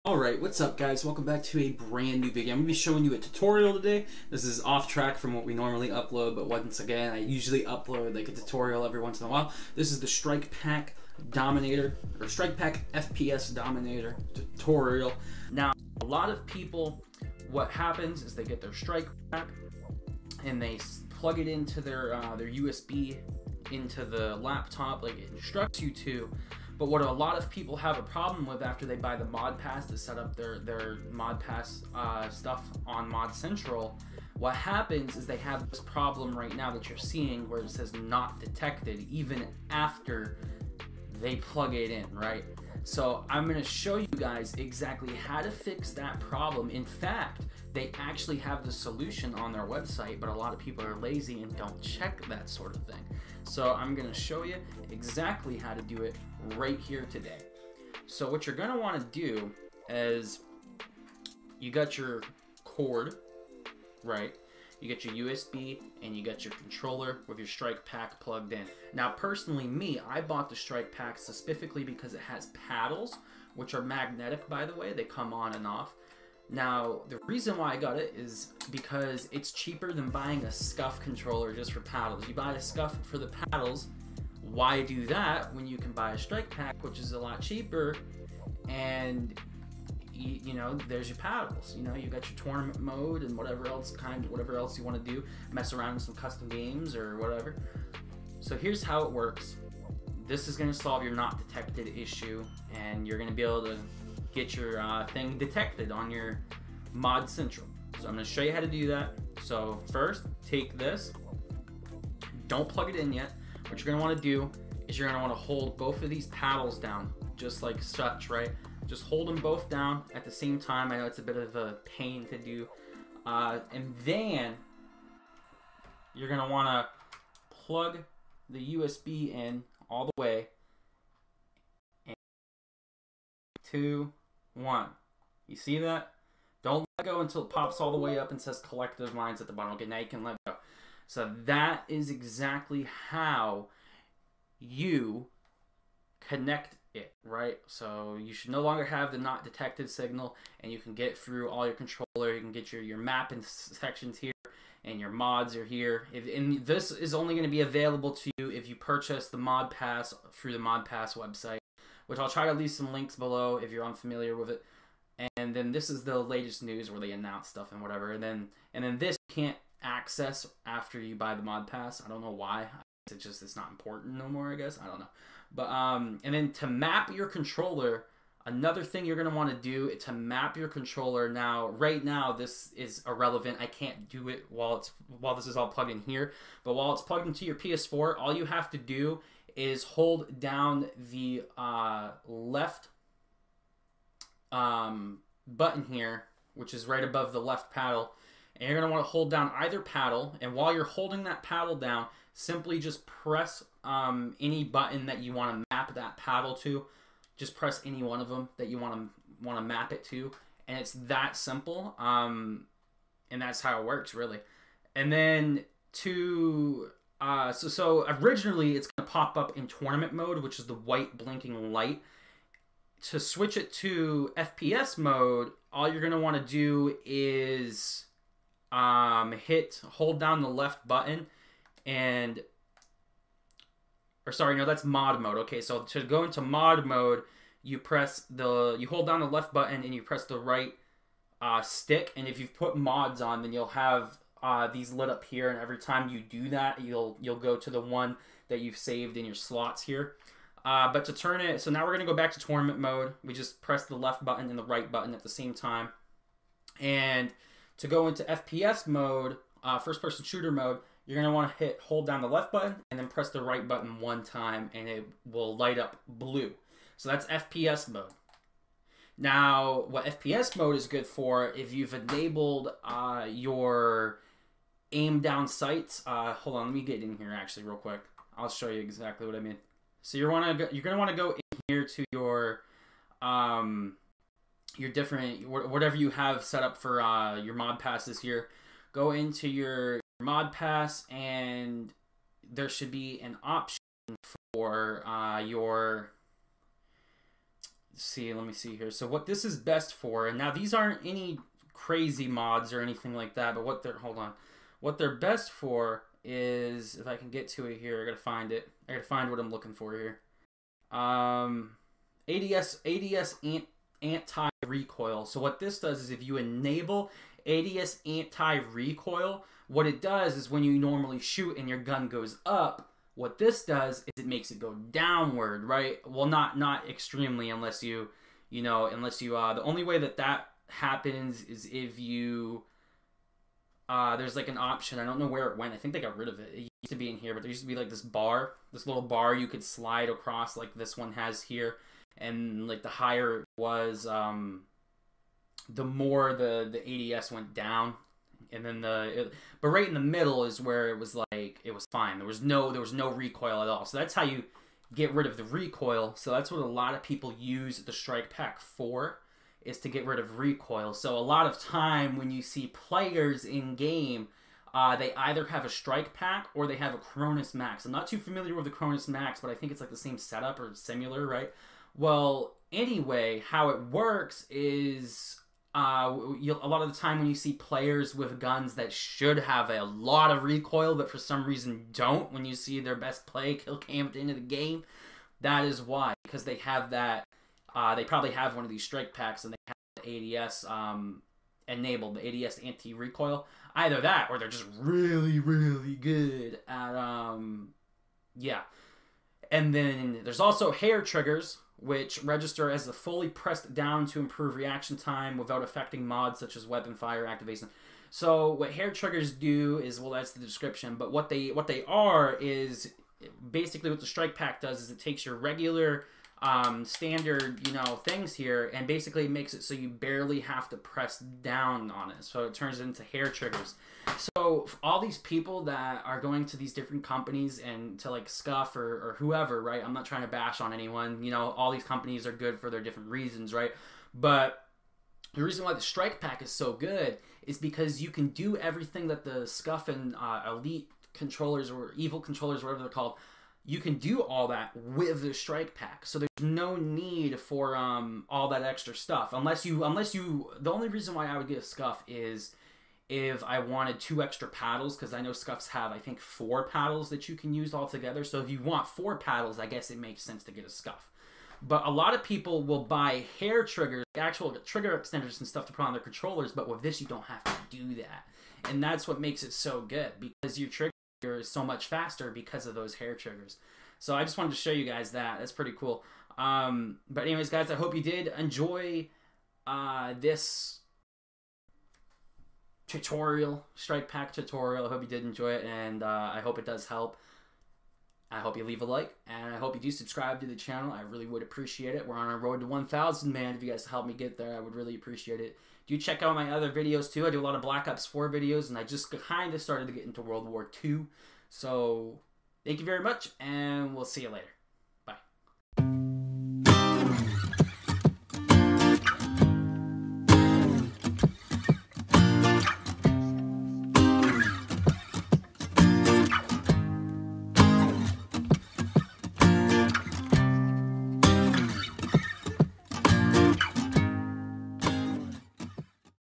Strikepack FPS Dominator NO DETECTION FIX 2022
I show you how to fix your Strikepack F.P.S Dominator NO DETECTION issues. Want to fix the "Not Detected" issue on Mod Central? Than watch this

Strikepack F.P.S Dominator NO DETECTION Fix 2022

How To Use Strikepack Mods (PS4 & Xbox One) 2022

Do I have a Twitter? Yes!